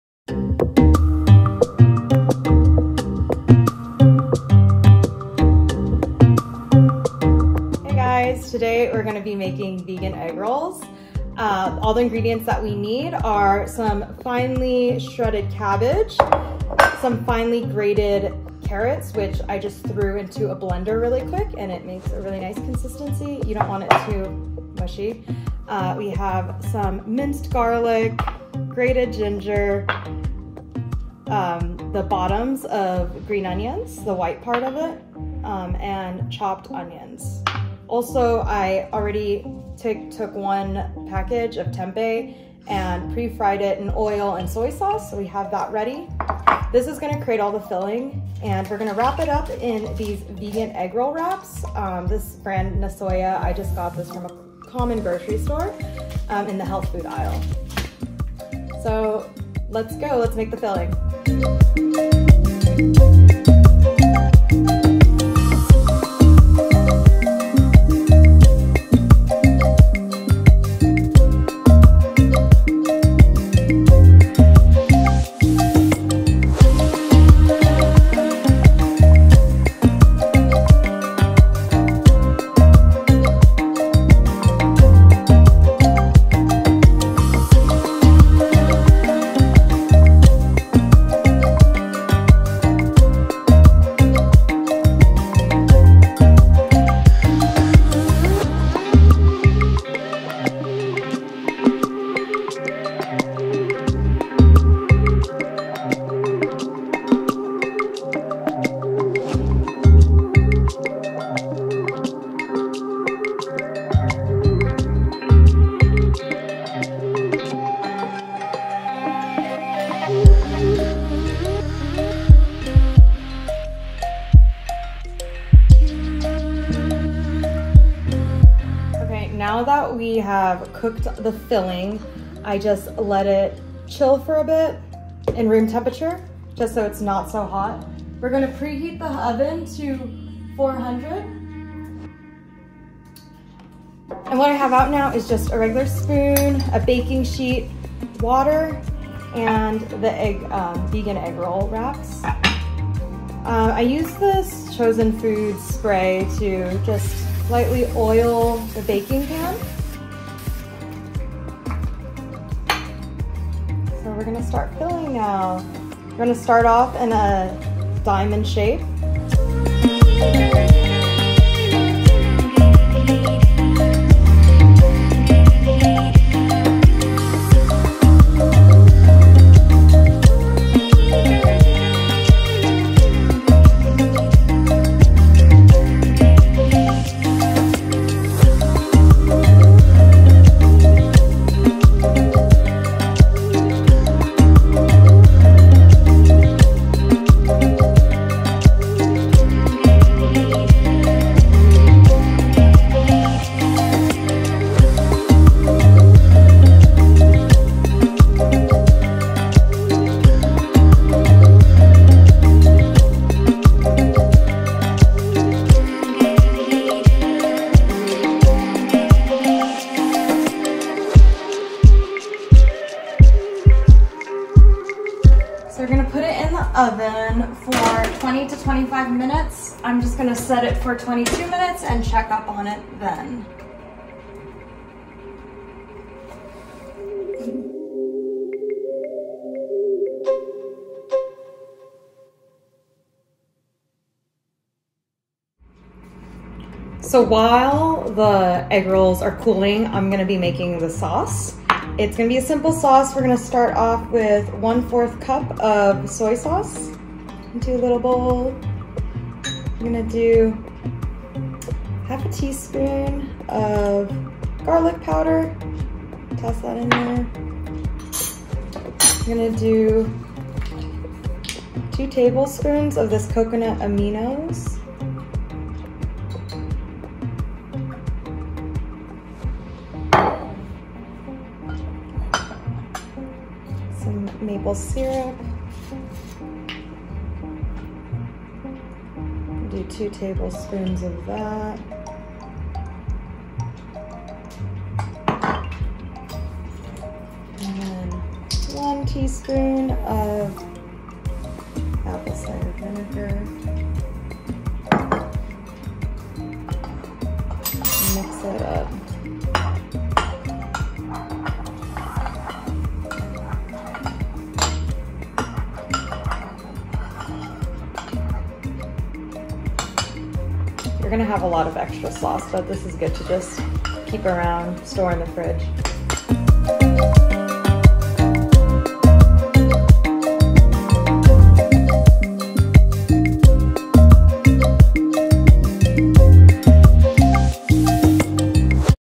Vegan Vegetable No-Egg Rolls
These plant-based (vegan) no-egg rolls are the perfect appetizer paired with a delicious dipping sauce.